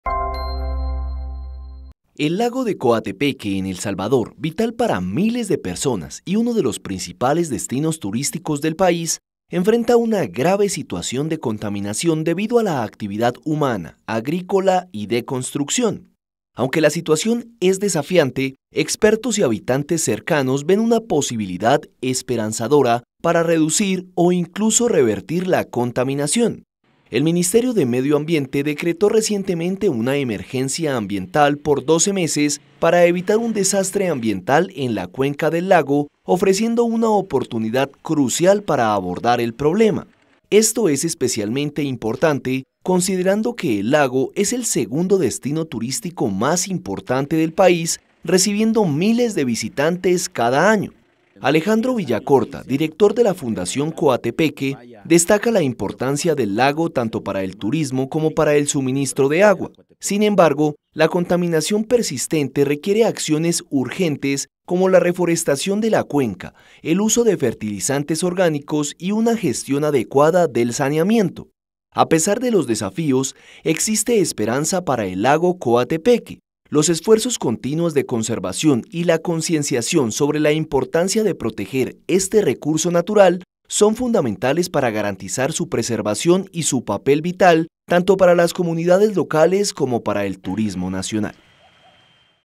El Salvador tiene la esperanzadora tarea de reducir la contaminación del lago de Coatepeque
Reducir o hasta revertir la situación de contaminación en la que se encuentra el lago de Coatepeque en El Salvador, uno de los principales destinos turísticos del país y proveedor de agua para unas 7.800 personas, es hoy por hoy una "posibilidad esperanzadora" para expertos y habitantes cercanos al cuerpo de agua.